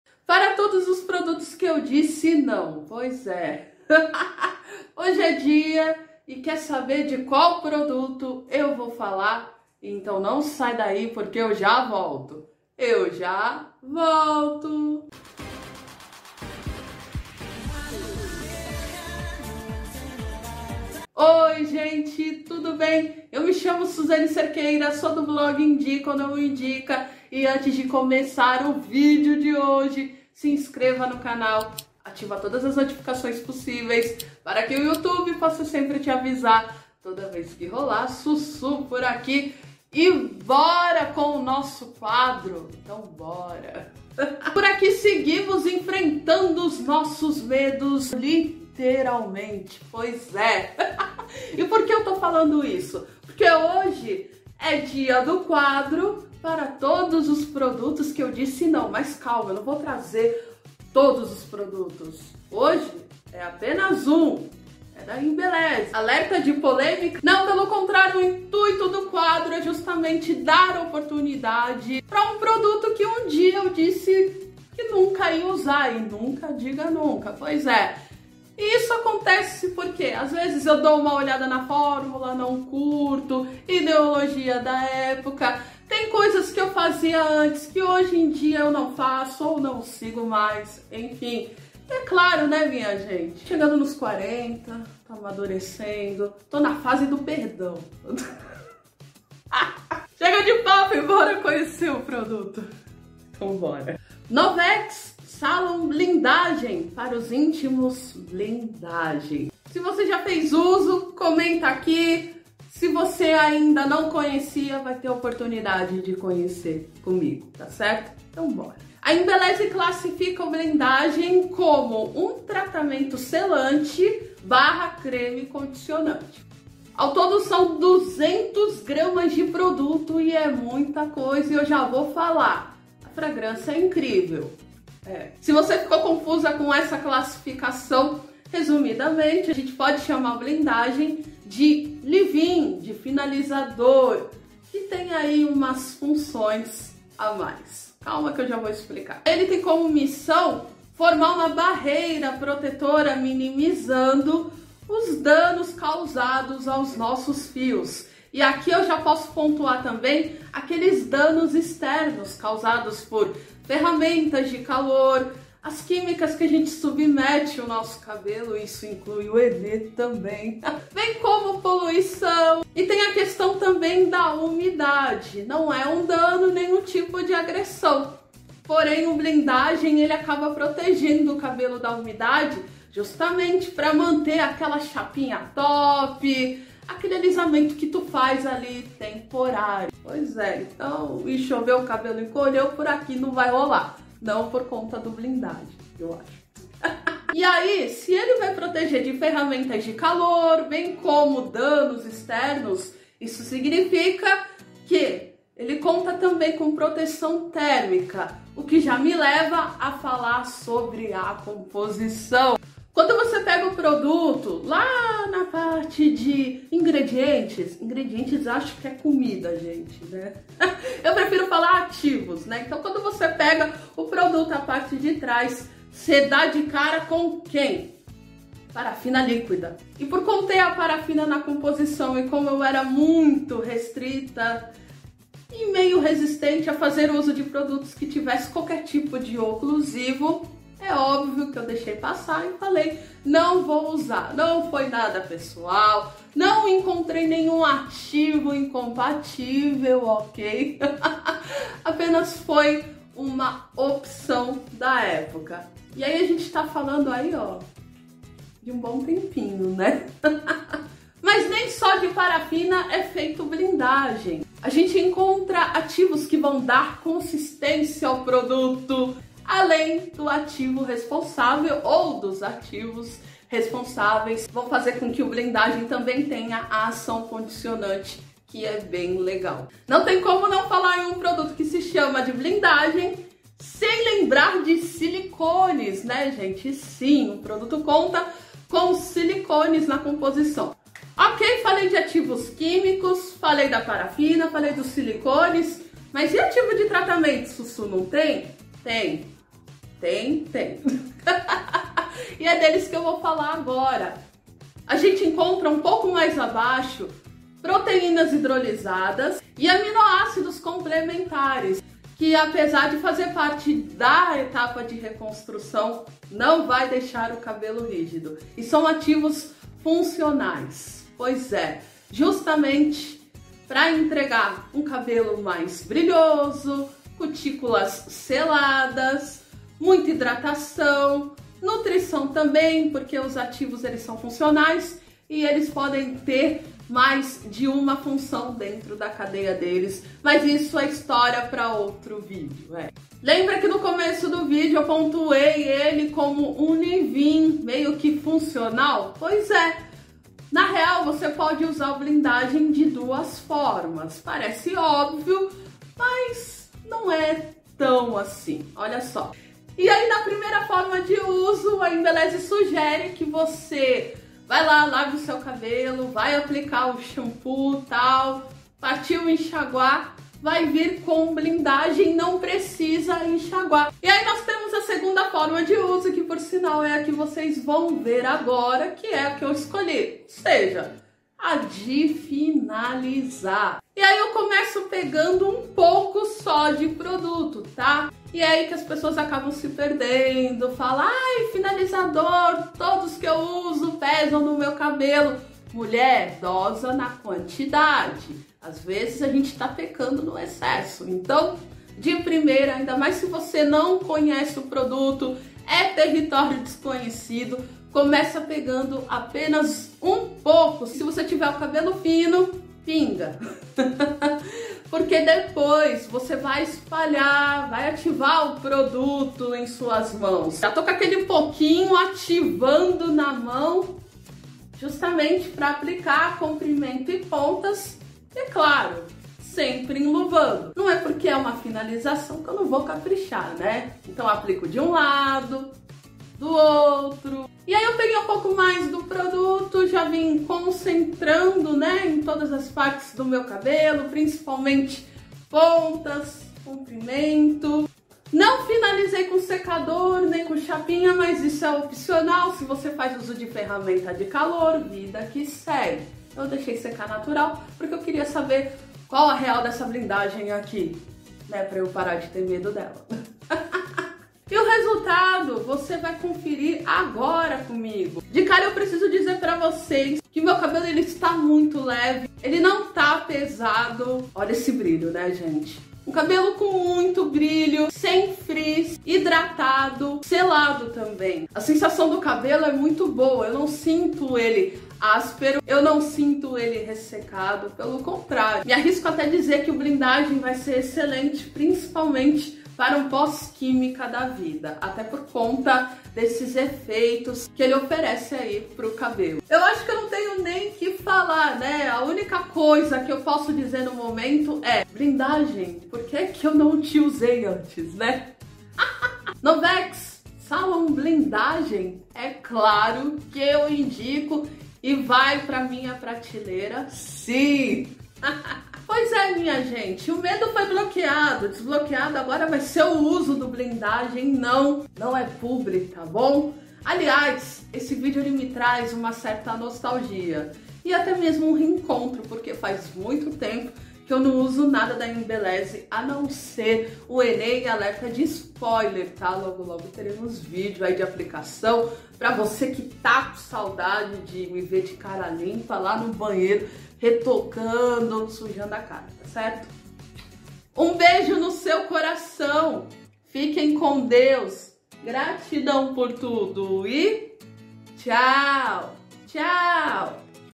2# Para todos os produtos que eu disse NÃO ❌
Tava com saudade dele? Então fica de boa porque ele ta de volta '
''Para todos os produtos que eu disse NÃO Novex Salon Blindagem tratamento selante/ creme condicionante da Embelleze, vem conferir!!

Se você estava com saudade também de ver Embelleze por aqui a hora é agora, só vem!

Ninguém pediu, mas caso precise Rainha minha tem cupom desconto para se jogar na loja online da Embelleze só acessar Cupom (suindica10)

Gratidão por tudo e até a próxima!